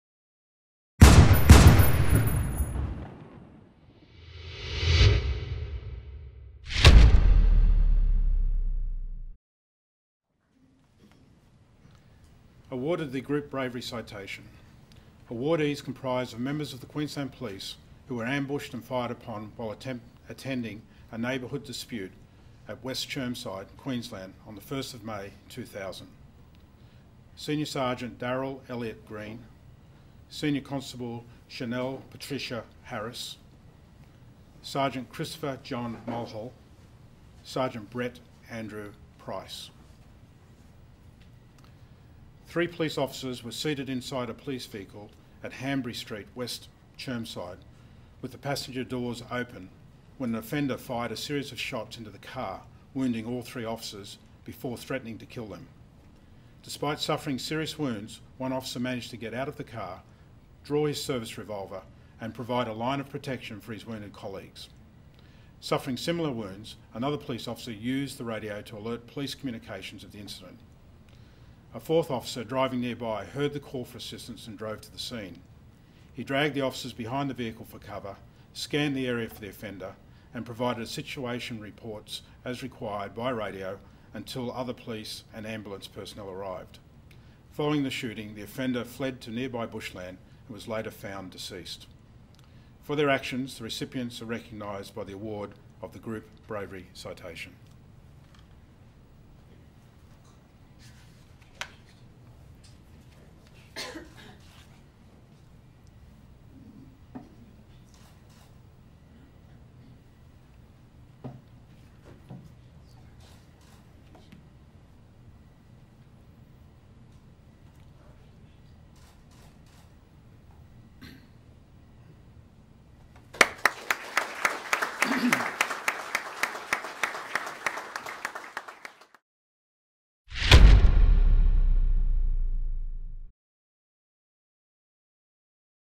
Investiture Ceremony, Group Bravery Citation Segment – Government House, 6 May 2016
On 6 May 2016 the Governor-General of the Commonwealth of Australia, with the authority of Her Majesty Queen Elizabeth The Second, Queen of Australia, awarded the Group Bravery Citation to Daryl Elliott Green, Sharnelle Patricia Harris, Christopher John Mulhall, and Brett Andrew Price, in recognition of a collective act in extraordinary circumstances.

The awardees comprise members of the Queensland Police who were ambushed and fired upon while attending a neighbourhood dispute at West Chermside, Queensland on 1 May 2000.

The Australian Bravery Decorations Council described the extraordinary circumstances as follows: ‘Three police officers were seated inside a police vehicle at Hanbury Street, West Chermside with the passenger doors open when an offender fired a series of shots into the car, wounding all three officers before threatening to kill them. Despite suffering serious wounds one office managed to get out of the car, draw his service revolver, and provide a line of protection for his wounded colleagues. Suffering similar wounds, another police officer used the radio to alert Police Communications of the incident. A fourth officer driving nearby heard the call for assistance and drove to the scene. He dragged the officers behind his vehicle for cover, scanned the area for the offender, and provided situation reports by radio until other police and ambulance personnel arrived. Following the shooting, the offender fled to nearby bushland and was later found deceased.’

This video shows the component of the investiture ceremony, where the four officers were awarded the Group Bravery Citation.